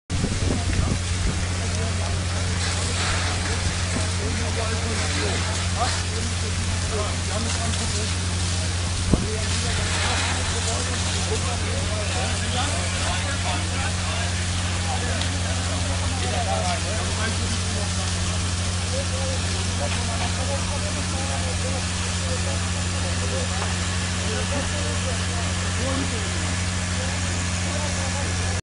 Scheune brennt in Ostermunzel (Barsinghausen), 24.03.2016
Copyright Bettina Richter, Calenberger Online News (CON)